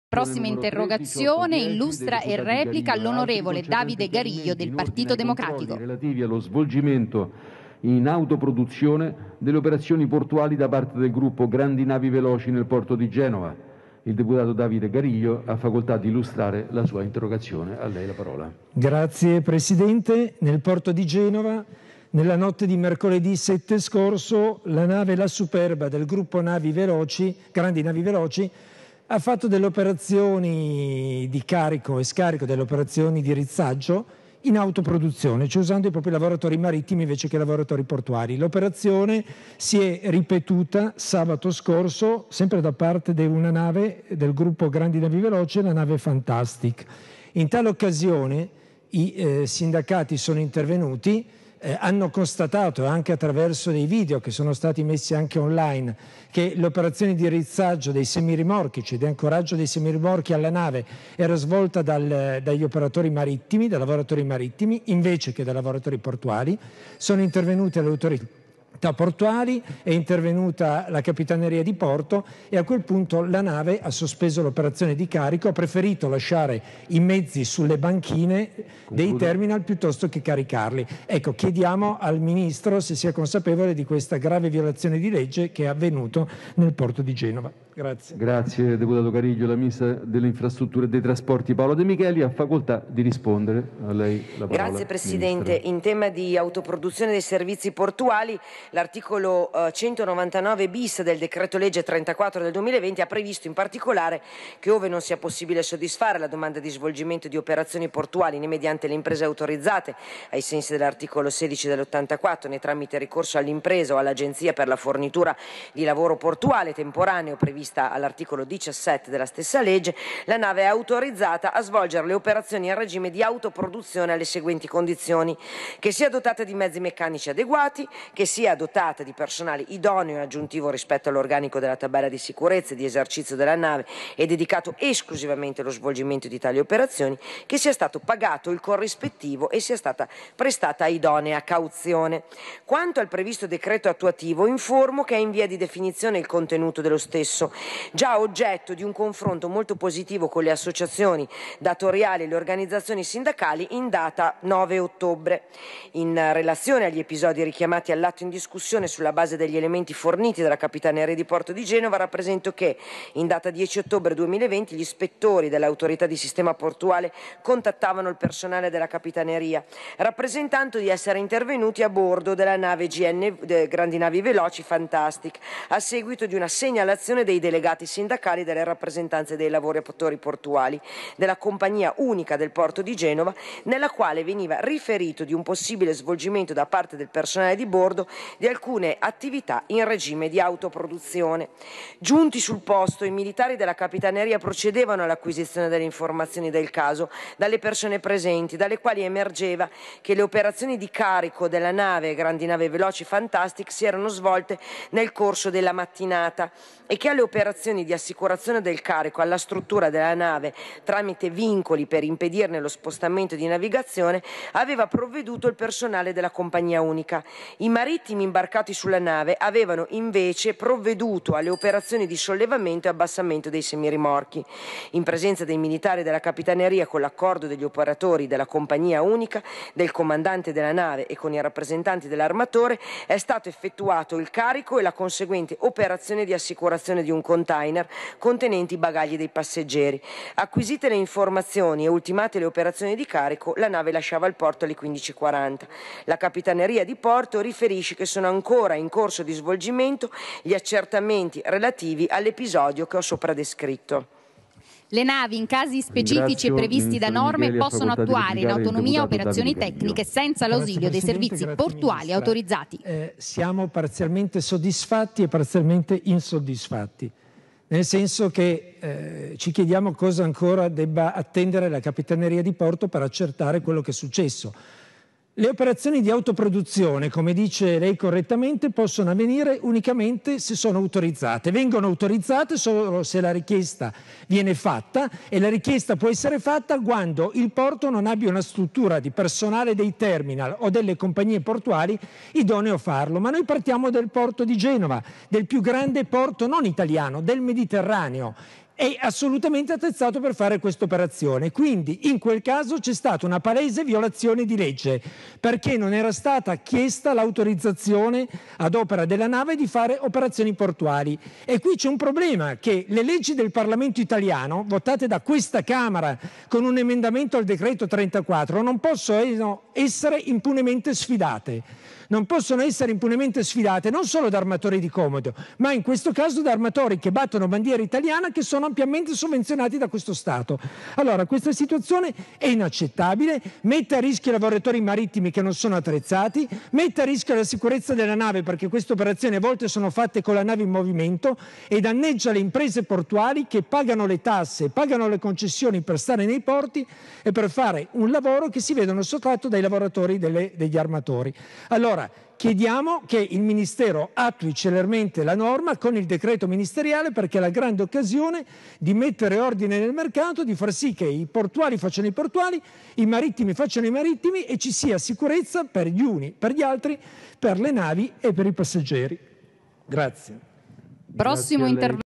Autoproduzione, l'insoddisfazione del Pd per la ministra Paola De Micheli
Arriva alla Camera il caso di autoproduzione di Gnv nel porto di Genova. Ed è scontro tra la ministra e il Pd sull'episodio.